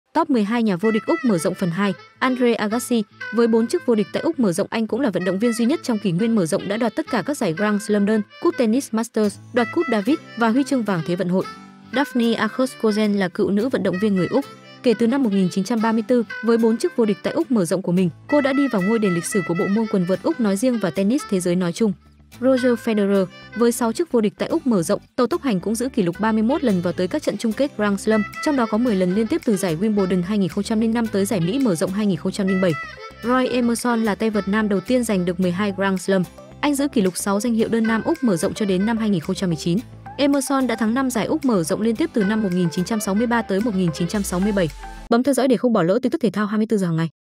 Top 12 Nhà Vô Địch Úc Mở Rộng (Phần 2) - Thể Thao 24H
Steffi Graf  là một cựu vận động viên quần vợt chuyên nghiệp người Đức. Cô đã giành được 4  chức vô địch đơn Úc mở rộng vào 3 năm liên tiếp 1988, 1989, 1980 và 1 cúp vào năm 1994 .
Evonne Goolagong là một cựu vận động viên quần vợt số 1 thế giới người Úc. Cô là một trong những tay vợt hàng đầu thế giới trong những năm 1970 và đầu những năm 1980. Cùng với Steffi Graf cô cũng đã giành được tổng cộng 4 chức vô địch Úc mở rộng.
 Ken Rosewall là một cựu tay vợt chuyên nghiệp và nghiệp dư hàng đầu thế giới đến từ Úc. Ông đã đạt được tổng cộng 4 chức vô địch Úc mở rộng và cũng tại đây  ông đã trở thành tay vợt nam đầu tiên trong kỷ nguyên mở để giành chiến thắng một giải đấu Grand Slam mà không thua một set nào.
Jack Crawford ông là một vận động viên quần vợt người Úc trong những năm 1930. Jack Crawford  cũng đã vô địch Úc mở rộng vào các năm 1931, 1932, 1933  và 1935. Anh ấy được giới thiệu vào Đài Danh vọng Quần vợt Quốc tế vào năm 1979.
Andre Agassi.  Với 4 chức vô địch tại Úc mở rộng  Anh cũng là vận động viên duy nhất trong Kỷ nguyên mở rộng đã đoạt tất cả các giải Grand Slam đơn, Cúp Tennis Masters, đoạt Cúp Davis, và huy chương vàng Thế vận hội.
Daphne Akhurst Cozens là cựu nữ vận động viên người Úc.  Kể từ năm 1934,  với 4 chức vô địch tại Úc mở rộng  của mình,  cô đã đi vào ngôi đền lịch sử của bộ môn quần vợt Úc nói riêng và tennis thế giới nói chung.
Roger Federer . Với 6 chức vô địch tại Úc mở rộng ,  Tàu tốc hành  cũng giữ kỷ lục 31 lần vào tới các trận chung kết Grand Slam, trong đó có 10 lần liên tiếp từ giải Wimbledon 2005 tới giải Mỹ mở rộng 2007.
Roy Emerson là tay vợt nam đầu tiên giành được 12 Grand Slam. Anh giữ kỷ lục sáu danh hiệu đơn nam Úc mở rộng cho đến năm 2019.  Emerson đã thắng năm giải Úc Mở rộng liên tiếp ( từ năm 1963 tới 1967).
Nancye Wynne Bolton là một vận động viên quần vợt người Úc. Cô đã sáu lần giành danh hiệu đơn nữ tại Giải vô địch Úc. Cô được vinh danh vào Đài danh vọng của quần vợt thế giới vào năm 2004.
Serena Williams với 7 chức vô địch tại Úc mở rộng , Hiện cô là tay vợt nữ hàng đầu của thế giới (có tổng số tiền thưởng cao nhất mọi thời đại). Cô cũng chính là tay vợt lớn tuổi nhất giành được danh hiệu Grand Slam.
Novak Djokovic. Ngày 29/1/2023, anh đã giành chức vô địch Úc mở rộng lần thứ 10 đồng thời trở lại ngôi số 1 thế giới để tiếp tục gia tăng kỷ lục số tuần đứng vị trí số một từ mốc 373 tuần.  Djokovic cũng là tay vợt giữ kỷ lục về tỷ lệ chiến thắng cao nhất lịch sử quần vợt.
Margaret Smith Court .Cô trở thành tay vợt nữ đầu tiên của Australia giành chiến thắng một giải Grand Slam ở nước ngoài, khi cô giành chiến thắng trong giải vô địch Pháp và Mỹ trong năm 1962. Các năm sau đó, cô tiếp tục giành chiến thắng tại Wimbledon và vô địch tổng cộng 11 lần tại Úc mở rộng vượt mặt Djokovic.